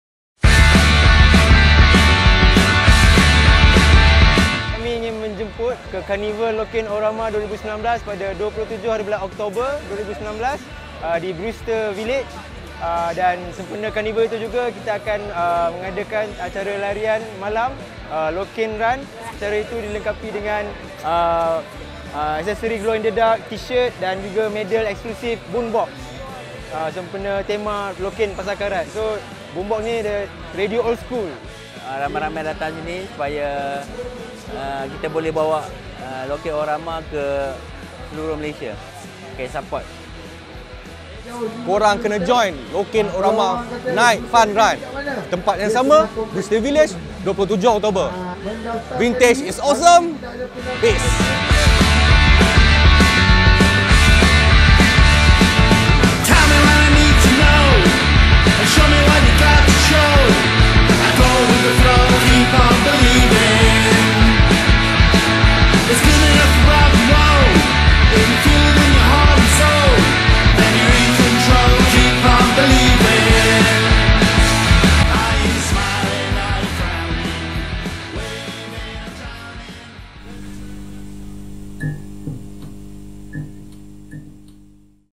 Lokein-O-Rama Night Fun Run 2019
for online registration.
EARLY BIRD TICKETS EXTENDED. LIMITED TICKETS AVAILABLE.

Fun run with nostalgic experience of vintage 1980s feel and ambience at Ipoh heritage old town! 😎😎

Event Details:-
Venue : Brewster Village, Jalan Sultan Idris Shah, Ipoh, Perak
Date : 27th October 2019 (Sunday)
*the next day (Monday) is public holiday (Deepavali)
Distance : 5km | MYR49 (Early Bird Price) *EXTENDED FOR LIMITED TICKETS ONLY.

Entitlement:
1. Lokein-O-Rama Exclusive T-Shirt
2. Exclusive Design Boombox Finisher Medal
3. FREE RM20 CASH VOUCHER from Lokein 😍😍
4. FREE RM5 CASH VOUCHER from Miker Pizza 😍😍
5. Refreshments